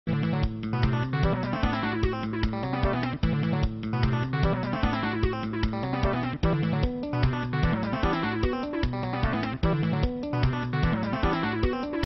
Line through a Given Point on a Given Plane Perpendicular to the Plane
Through a given point in a given plane there is one and only one line perpendicular to the plane.

Contributed by: Heather Byrne